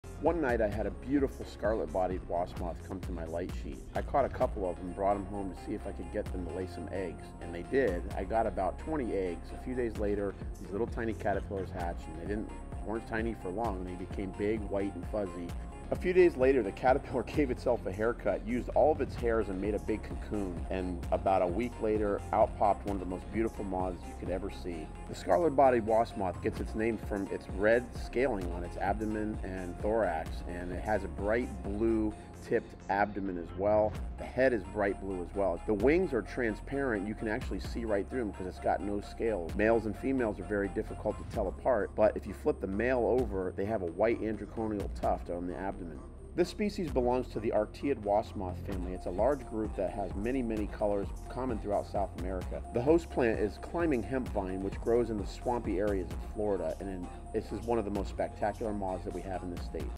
BEAUTIFUL RED MOTH - Scarlet Bodied Wasp Moth Cosmosoma myrodora
This short video will show you the lifecycle of one of the most beautiful mods in Florida. The Scarlet, bodied, wasp moth, Cosmosoma myrodora, is a day flying Arctiid moth that looks very much like a wasp. It uses mimicry to protect itself against predators. It feeds on climbing hemp vine as a marvel host plant and can be found in many habitats in the state of Florida.